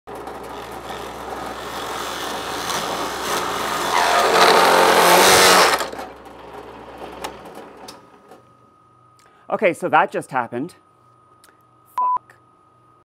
The wrong tool for the job
Don't use construction-grade spade bits in a pillar drill, children...